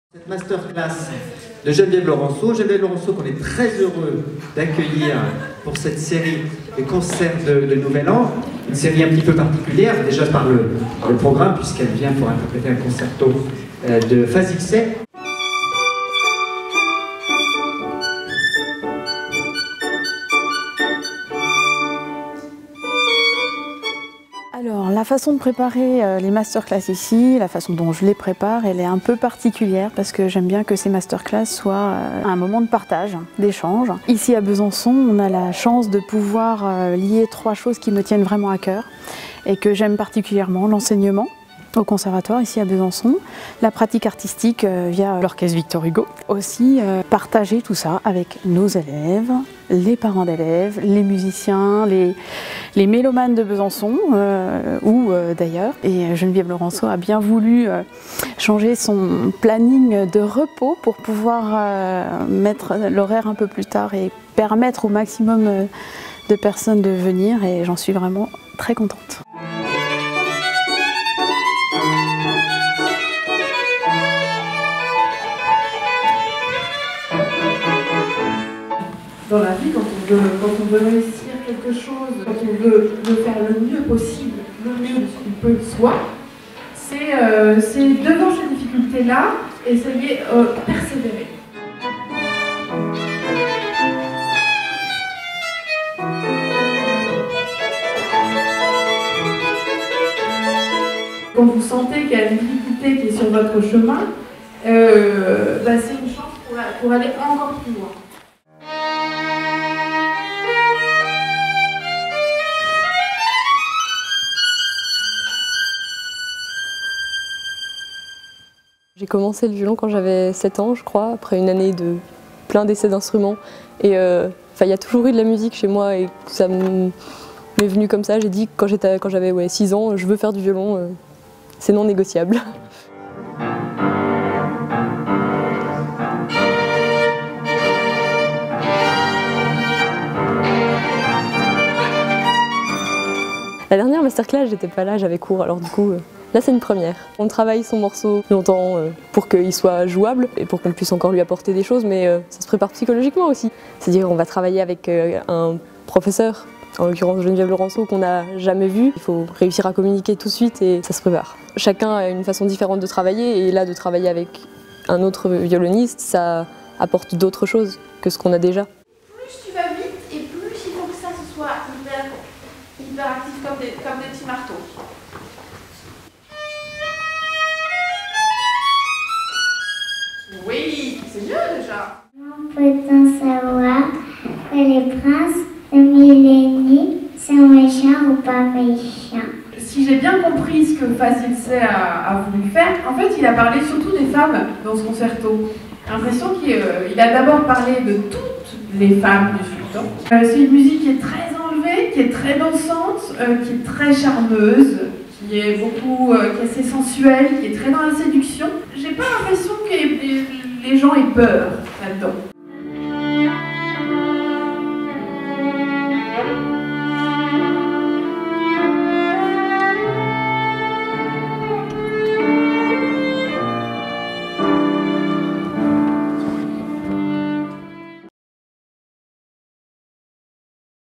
Geneviève Laurenceau partage son énergie avec les étudiants du Conservatoire
MASTER-CLASS avec Geneviève Laurenceau, violoniste française
en partenariat avec l'Orchestre Victor Hugo Franche-Comté et le CRD de Montbéliard

Conservatoire à Rayonnement Régional [CRR] du Grand Besançon
Enseignement artistique spécialisé en MUSIQUE, DANSE et THEATRE